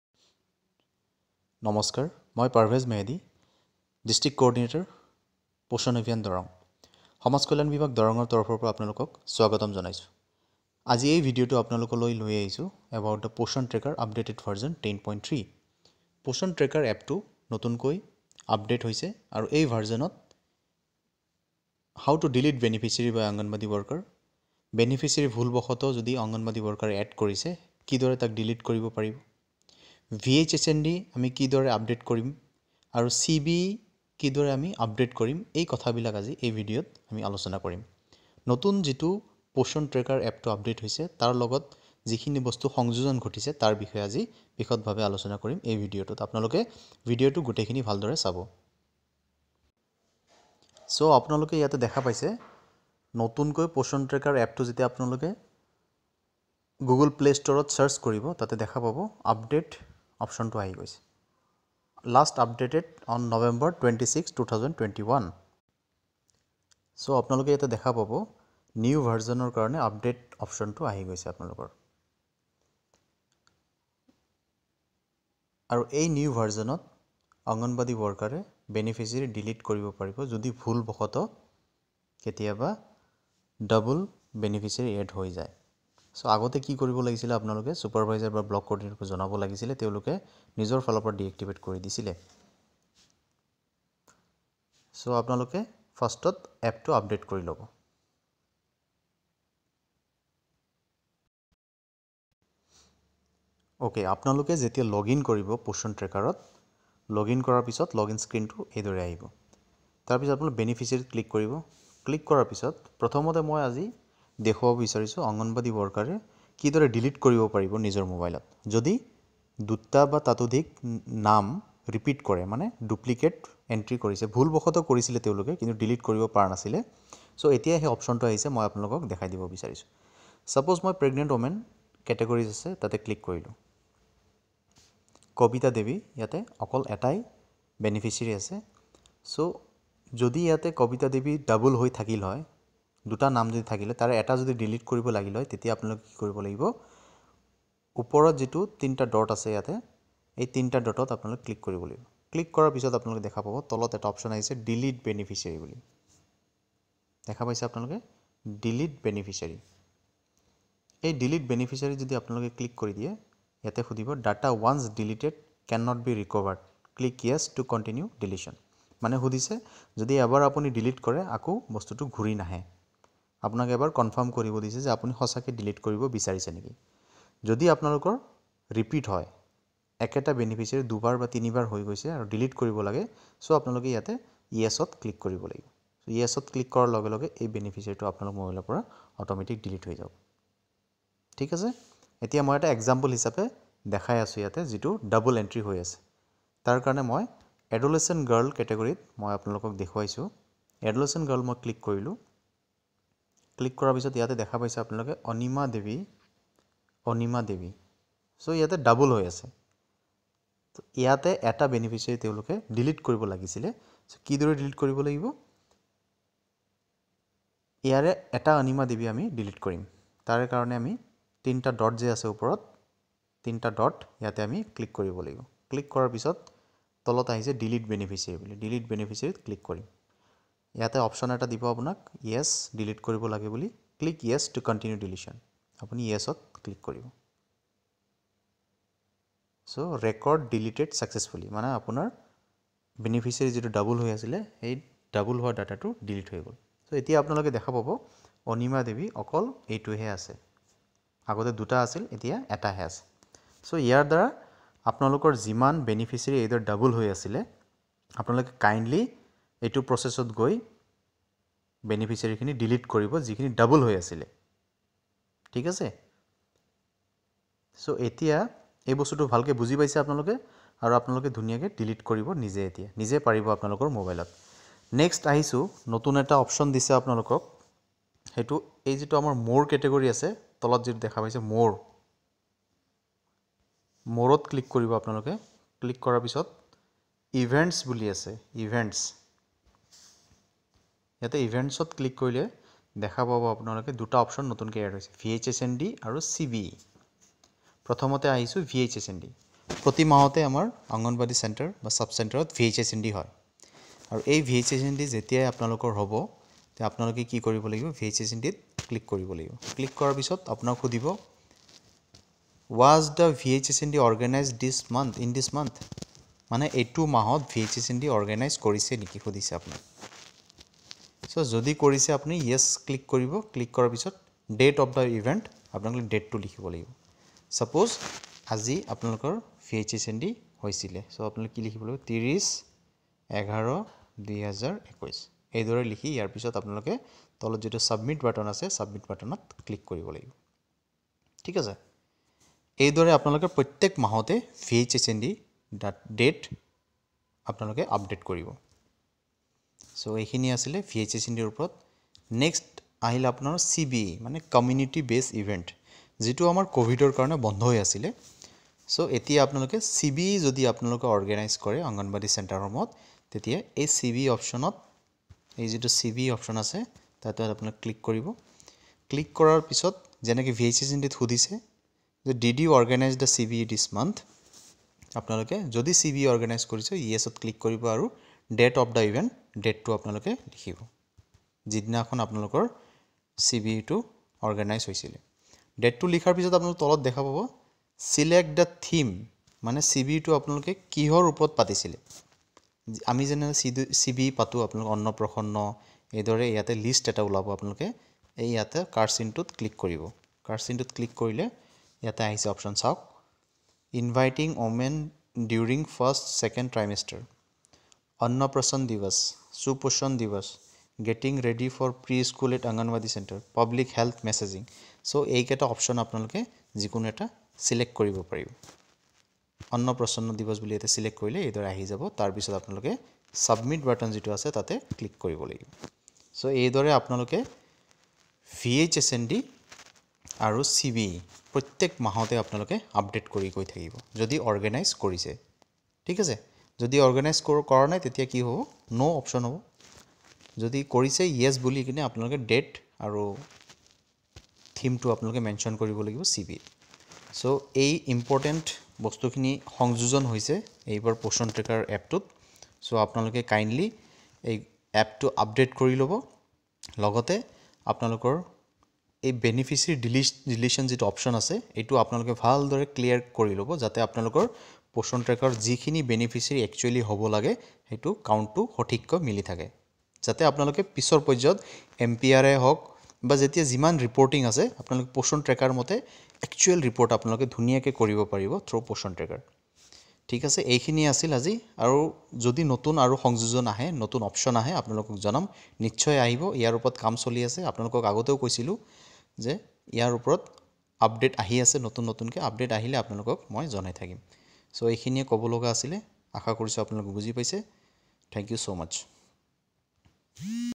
Update V 10.3 || How to delete beneficiary from POSHAN Tracker App by AWW || VHSND ?? CBE || Darrang
হিতাধিকাৰী কিদৰে‌ Delete কৰিব?? পোষণ ট্ৰেকাৰত ভুলতে ডাবল এন্ট্ৰি কৰিলে কি কৰিব??   How to delete beneficiary from POSHAN Tracker App || AWW || VHSND ?? CBE ?? || Darrang